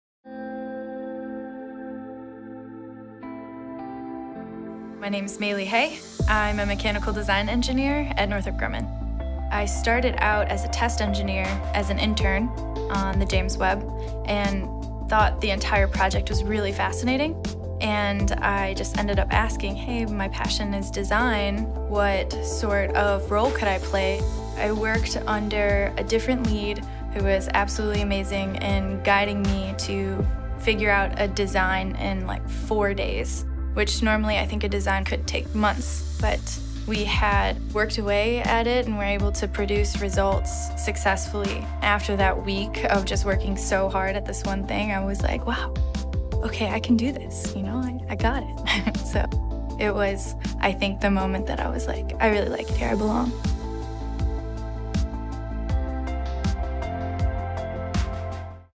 Mei-Li is Defining Possible with Northrop Grumman
From intern to mechanical design engineer, Mei-Li is Defining Possible every day at Northrop Grumman.